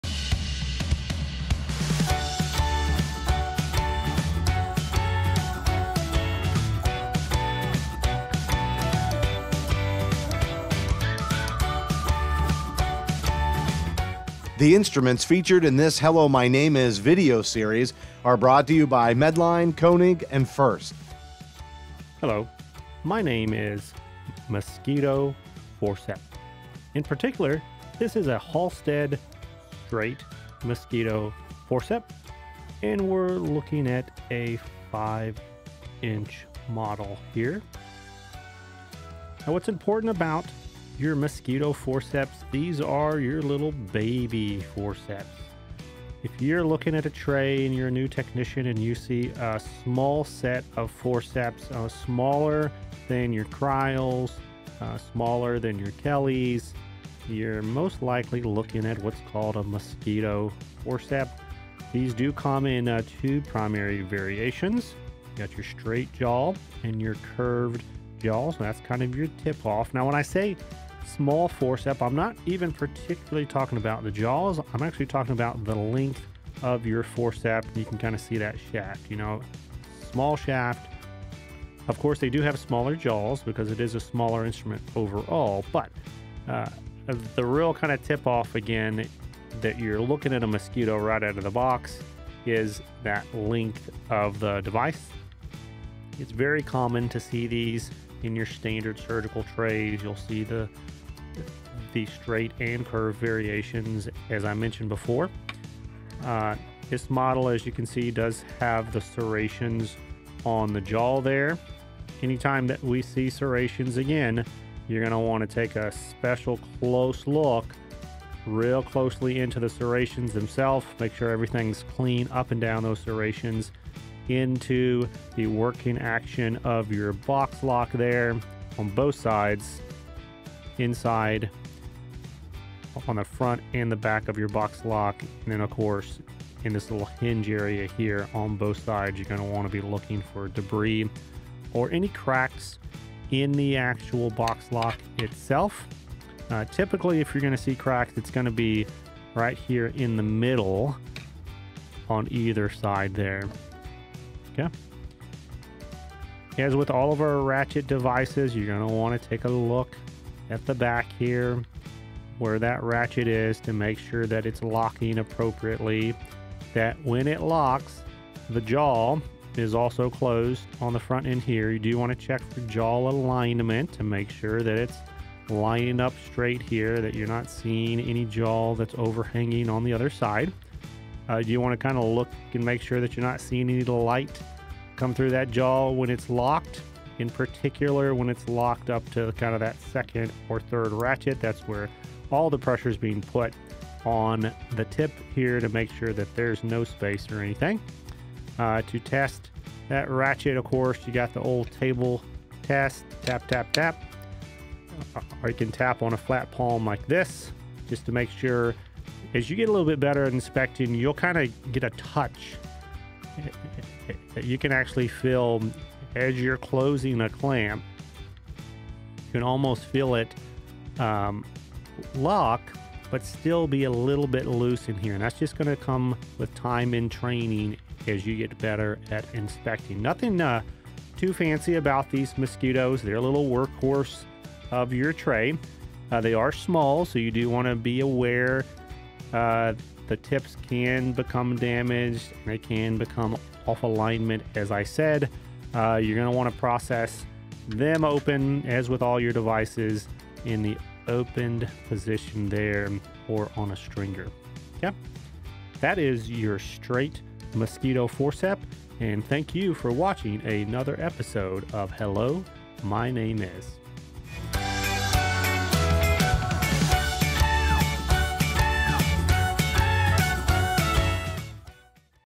Hello, My Name is: Halsted-Mosquito Forcep | Surgical Instruments | Beyond Clean
Welcome to the "Hello, My Name Is..." educational video series, where we discuss surgical identification, tips, & tricks for Sterile Processing professionals from industry insider and thought leader, Hank Balch. On this episode we'll be discussing the Halsted Mosquito Forcep. Grab your favorite instrument and tune in for a quick introduction to the instruments most likely to come across your reprocessing table in the days to come!

For more information on this instrument and other Sterile Processing products, check out our series sponsor, Medline at: